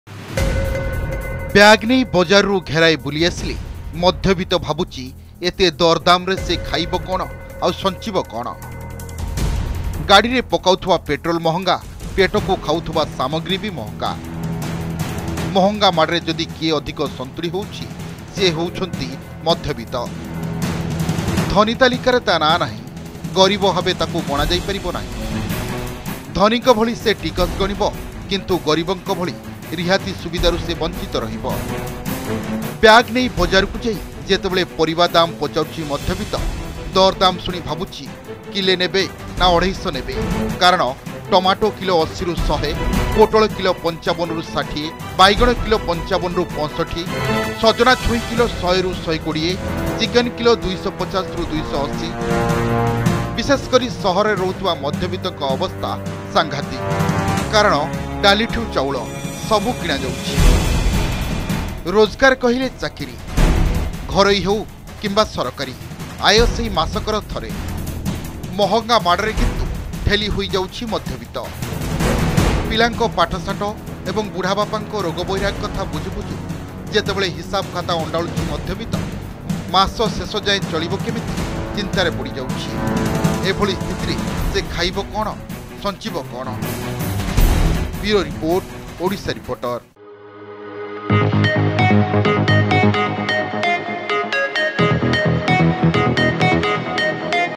ମଧ୍ୟବିତ୍ତ…ସଞ୍ଚିବ କ’ଣ? ଖାଇବ କ’ଣ ? | Odisha Reporter
ବ୍ୟାଗ୍ ନେଇ ବଜାରରୁ ଘେରାଏ ବୁଲି ଆସିଲେ, ମଧ୍ୟବିତ୍ତ ଭାବୁଛି ଏତେ ଦରଦାମରେ ସେ ଖାଇବ କ’ଣ ଆଉ ସଞ୍ଚିବ କ’ଣ? ଗାଡ଼ିରେ ପକାଉଥିବା ପଟ୍ରୋଲ ମହାଙ୍ଗା, ପେଟକୁ ଖାଉଥିବା ସାମଗ୍ରୀ ମହଙ୍ଗା। ମହଙ୍ଗା ମାଡ଼ରେ ଯଦି କିଏ ଅଧିକ ସନ୍ତୁଳି ହେଉଛି, ସିଏ ହେଉଛି ମଧ୍ୟବିତ୍ତ।